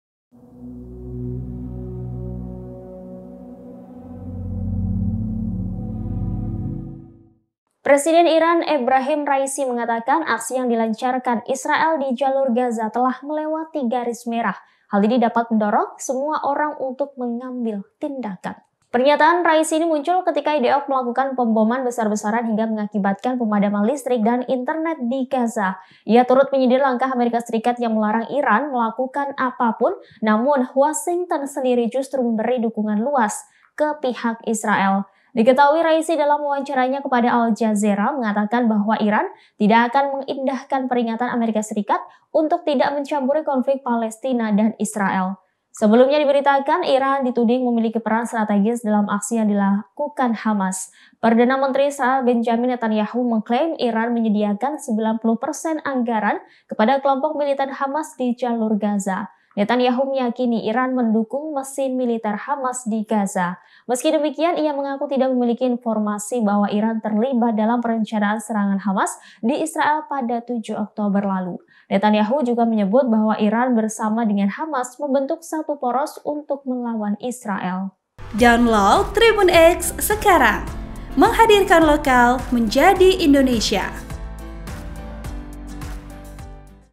Sindir Keras AS, Presiden Iran: Kejahatan Israel Lewati 'Garis Merah' yang Bisa Picu Dunia Marah
TRIBUN-VIDEO.COM - Presiden Iran Ebrahim Raisi  mengatakan aksi yang dilancarkan Israel di Jalur Gaza telah melewati garis merah.

Hal ini dapat mendorong semua orang untuk mengambil tindakan.

Pernyataan Raisi ini muncul ketika IDF melakukan pemboman besar-besar hingga mengakibatkan pemadaman listrik dan internet di Gaza.

Ia turut menyindir langkah AS yang melarang Iran melakukan apapun, namun Washington justru memberi dukungan luas ke Israel.

Diketahui, Raisi dalam wawancaranya kepada Al Jazeera mengatakan bahwa Iran tidak akan mengindahkan peringatan AS untuk tidak mencampuri konflik Palestina-Israel.

Sebelumnya diberitakan, Iran dituding memiliki peran strategis dalam aksi yang dilakukan Hamas.

PM Israel Benjamin Netanyahu mengklaim Iran menyediakan 90 persen anggaran kepada Kelompok militan Hamas di Jalur Gaza.

Netanyahu meyakini Iran mendukung mesin militer Hamas di Gaza.

Meski demikian, ia mengaku tak memiliki informasi bahwa Iran terlibat dalam perencanaan serangan Hamas di Israel pada 7 Oktober lalu.

Netanyahu juga menyebut bahwa Iran bersama dengan hamas membentuk satu poros untuk melawan Israel.

(Tribun-Video.com)

Vp: Ulung